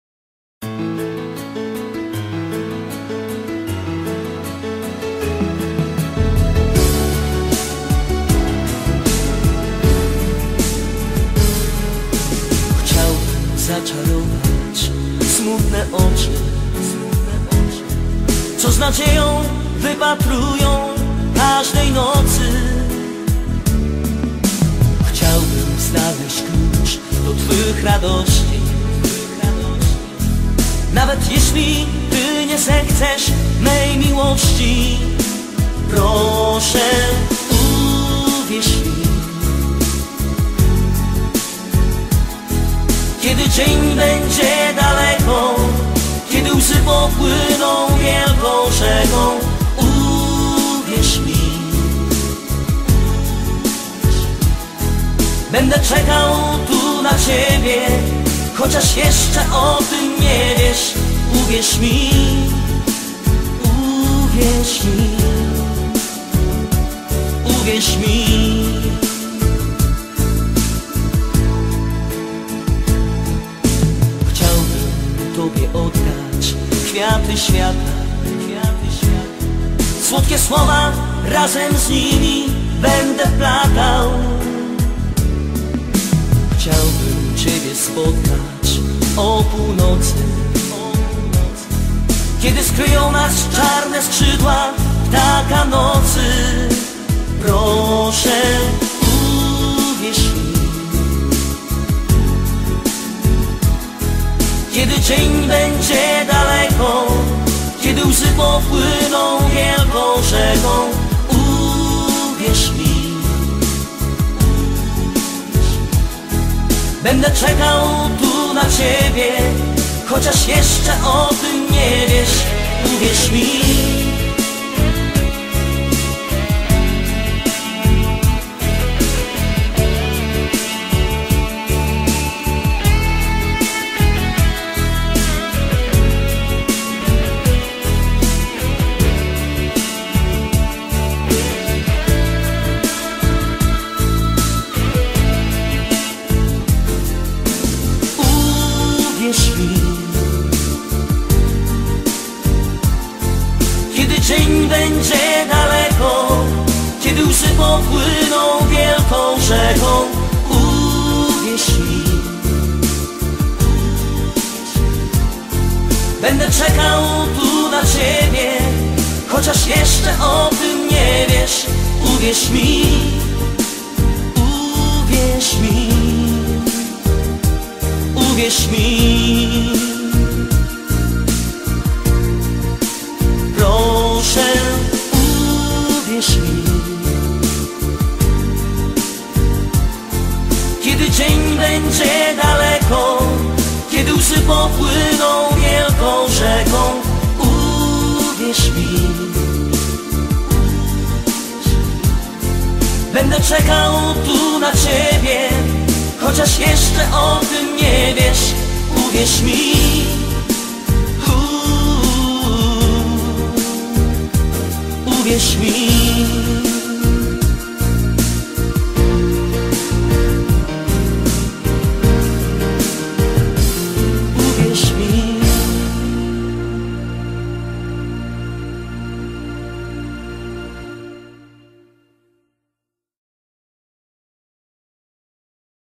💖TU NA CIEBIE BĘDĘ CZEKAŁ💖
Tak bardzo Cię pragnę
Lecz jesteś dla mnie niedostępna.
Uwięziona w miłości
Pozbawionej sensu i piękna.
Będę na Ciebie czekał
Miłością którą kryje moje serce
Kiedyś przepędzę te wszystkie czarne chmury.
I razem popłyniemy ku dolinie szczęścia
Związani miłością
Która nigdy nie złamie nam serca